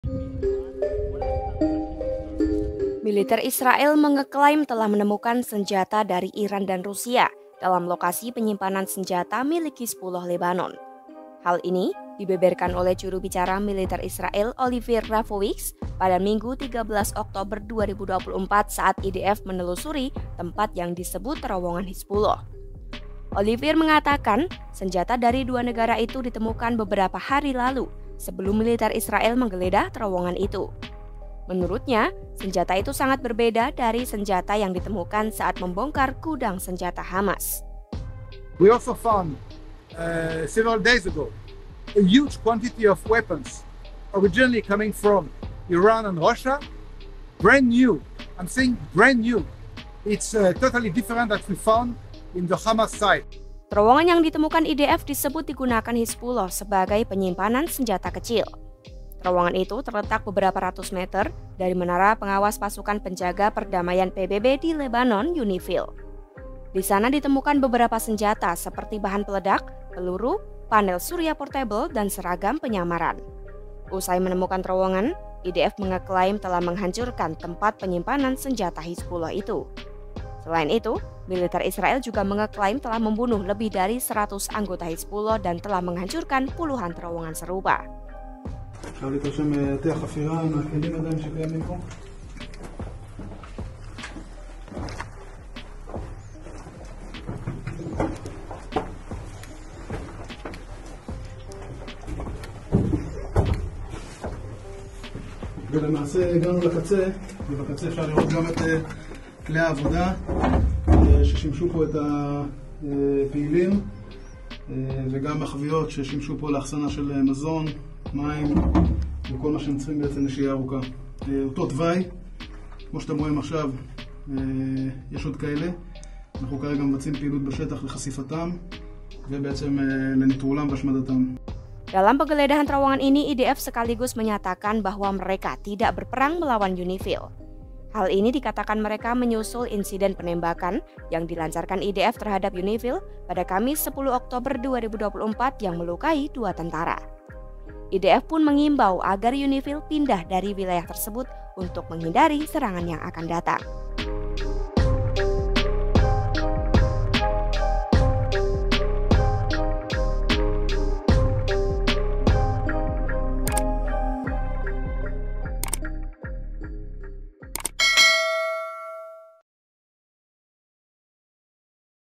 Tunjukkan Terowongan Hizbullah, Militer Israel Sebut Temukan Senjata dari Iran dan Rusia
Militer Israel mengeklaim telah menemukan senjata dari Iran dan Rusia dalam lokasi penyimpanan senjata milik Hizbullah Lebanon. Hal ini dibeberkan oleh Juru Bicara Militer Israel Olivier Rafowicz, pada Minggu (13/10/2024) saat IDF menelusuri tempat yang disebut terowongan Hizbullah.

Pihaknya mengatakan, senjata dari dua negara itu ditemukan beberapa hari lalu sebelum IDF menemukan terowongan Hizbullah. Menurutnya, senjata tersebut sangat berbeda dari senjata yang IDF temukan saat membongkar gudang senjata Hamas.

Adapun terowongan yang ditemukan IDF disebut digunakan Hizbullah sebagai penyimpanan senjata kecil yang terletak beberapa ratus meter dari menara pengawas pasukan UNIFIL. Di sana, militer Israel mengaku menemukan senjata seperti bahan peledak, peluru, panel surya portabel, dan seragam penyamaran.

Penulis Naskah: Adinda Septia Berliana
Narator: Adinda Septia Berliana
Video Editor: Maria Utari Dewi
Produser: Deta Putri Setyanto (Farid)